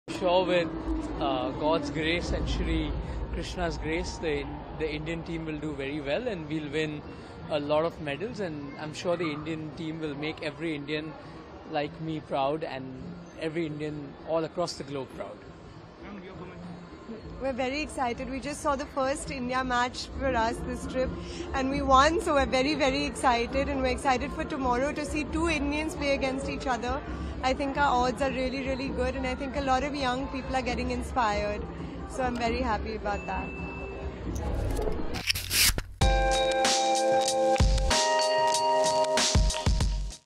'I am sure the Indian team will make every Indian like me proud' says Anant Ambani at Paris Olympics
Reliance Industries Chairman Mukesh Ambani's son Anant Ambani at Paris Olympics, 'I am sure that with god's grace, the Indian team will perform very well and we will win many medals. I am sure the Indian team will make every Indian like me proud'. His wife Radhika Merchant further added that,'We are very excited because we watched the first India match and we won, which made it even more exciting... Our odds are really good and a lot of young people are getting inspired'.